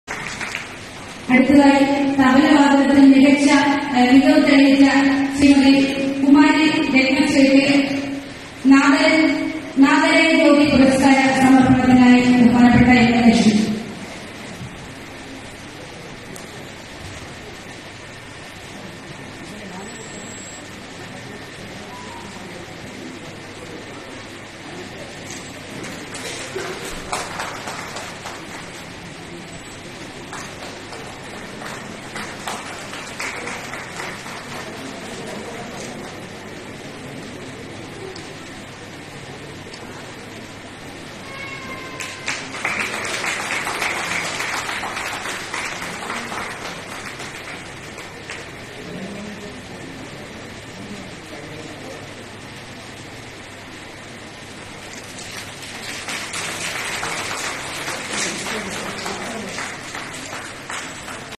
Award function @Mathangi school of music 2022 April 10th Sri rama navami day
Retnasree Receiving "Nadalaya jyoti "Purasskaram 2022 from Sri.Thiruvanchur Radhakrishnan @Mathangi school of Music kottayam on Steeramanavami day April 10th 2022.